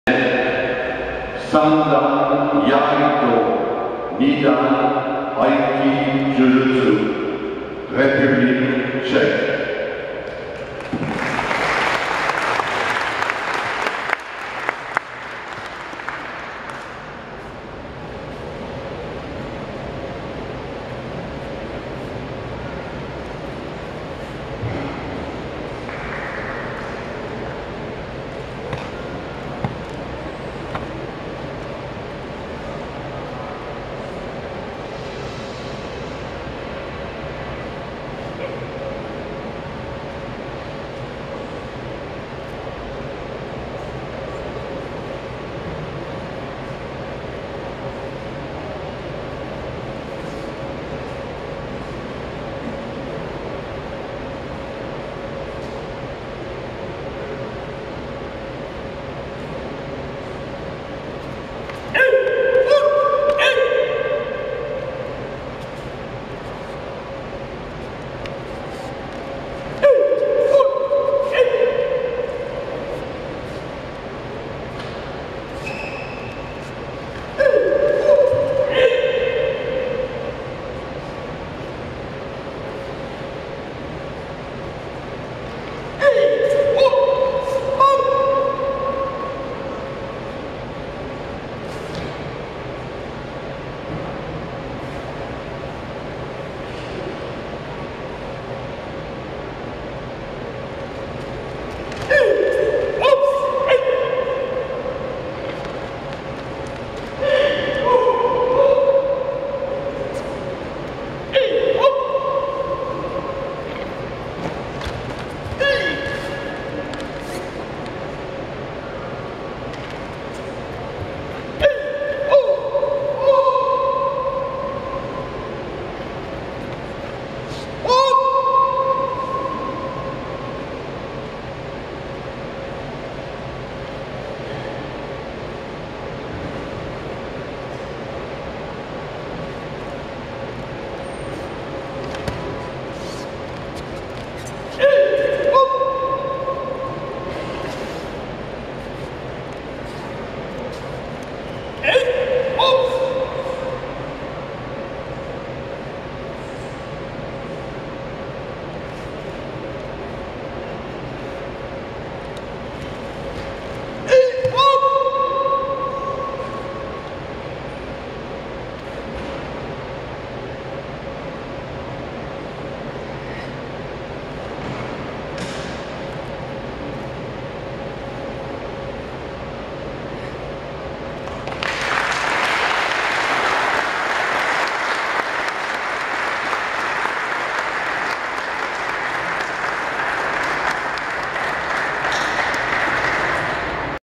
Taikai 2019 - Filip MANEK (3 dan Iai-do, Czech)
The demonstration presented by Filip MANEK (3 dan Iai-do, Czech) on "First European Taikai Aiki-Jujutsu Takeda-Ryu MAROTO-HA", Alexandria, Romania, 14-16 June 2019